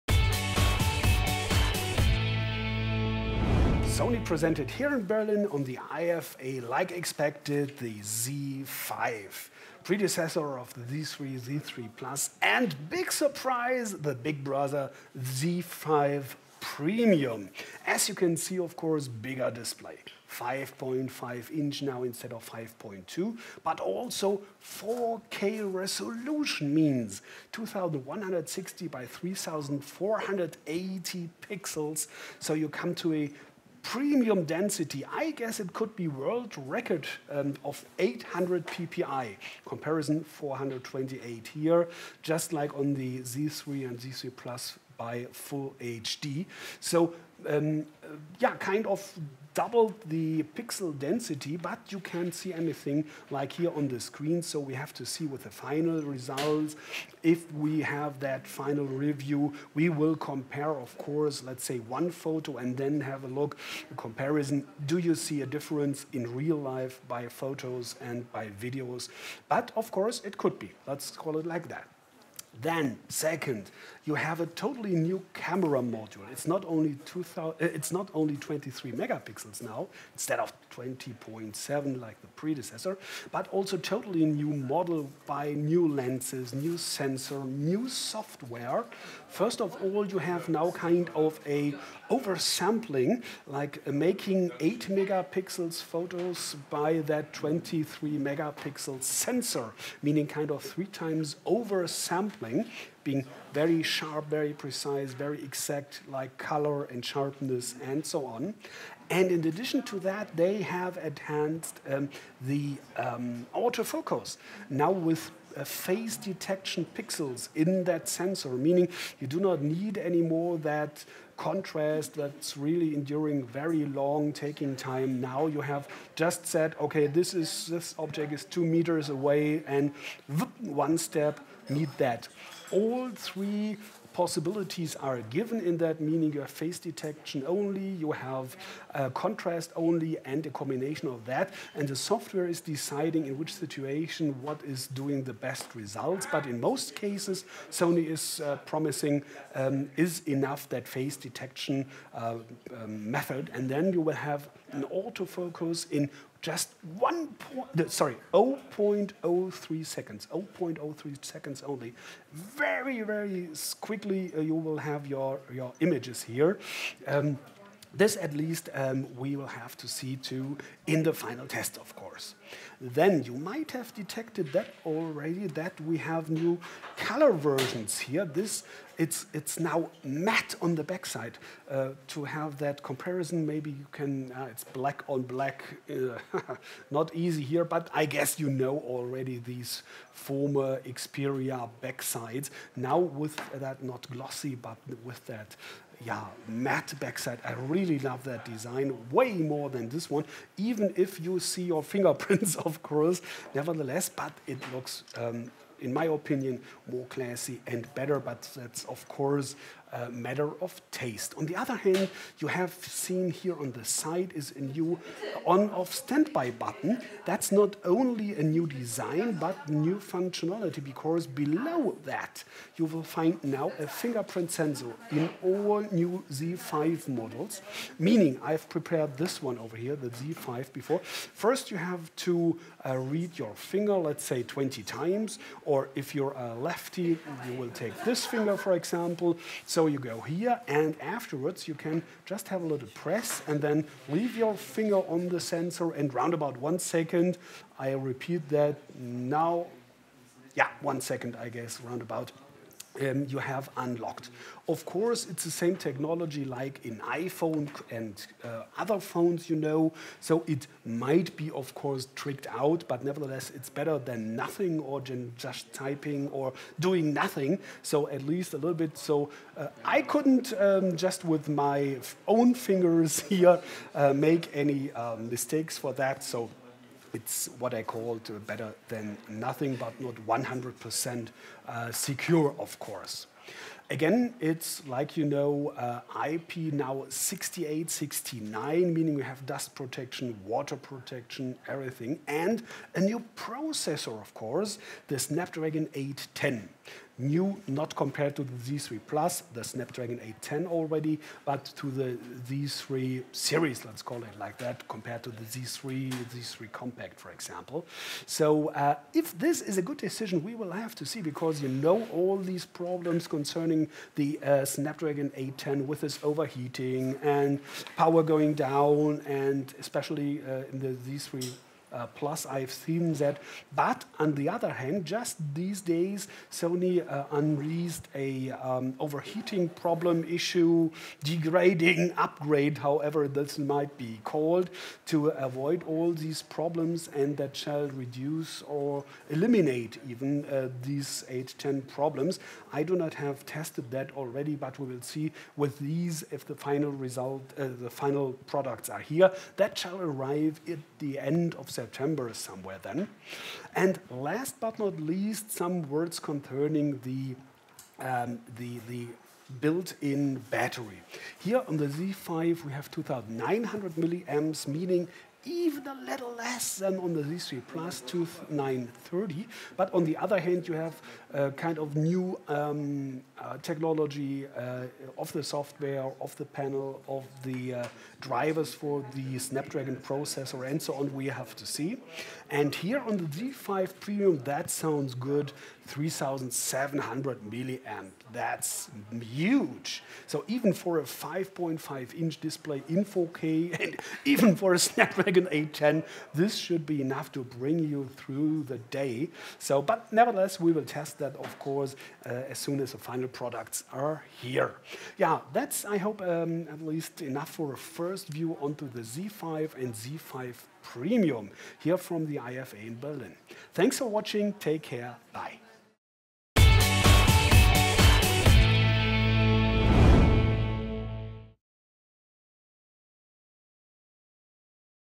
Sony Xperia Z5 + Z5 Premium: First 4K Screen Phone — Video Review
Sony showed on the IFA in Berlin not only a new Z5, but surprised also with a Z5 Premium. That bringt for the first time a 4K display (2160 x 3840 pixels), coming to a record sharpness of 800 ppi! This hands-on video shows both phones, explains the differences and what has changed compare to the Z3+.

What do you think of the Z5 and Z5 Premium? Which one is your favorite and why? Leave your opinion in the comments!